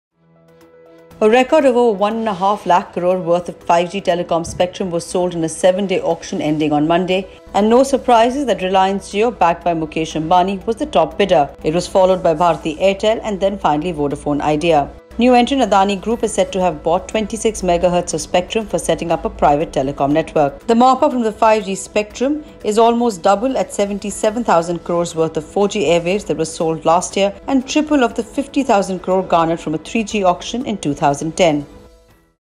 5G auction: Bids cross record ₹1.5 lakh cr mark
A record over Rs 1.5 lakh crore worth of 5G telecom spectrum was sold in a seven-day auction ending on Monday, which according to the reports, puts the total bids at Rs 1,50,173 crore. Reliance Jio was the top bidder. It was followed by Bharti Airtel and Vodafone Idea Ltd.